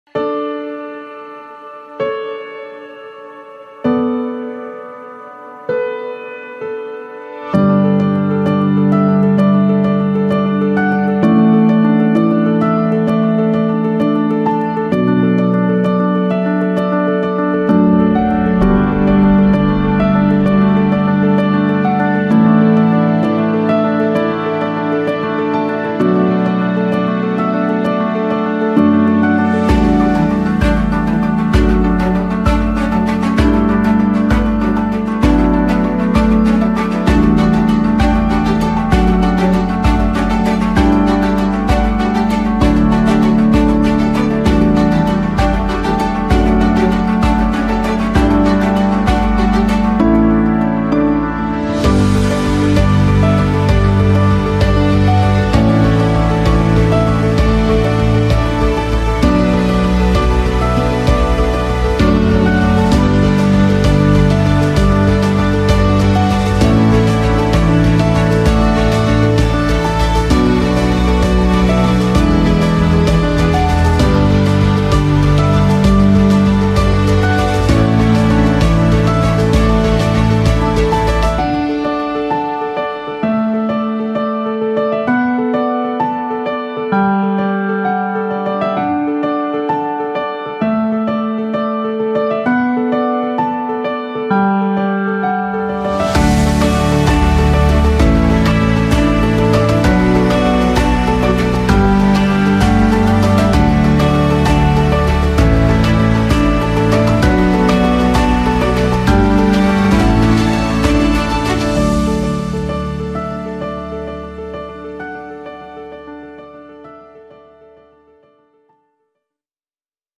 WE SAID YES ! VERLOBUNGSTRAILER ARDIANA & NALI
Unsere Verlobungsfeier im Seerestaurant in Rorschach !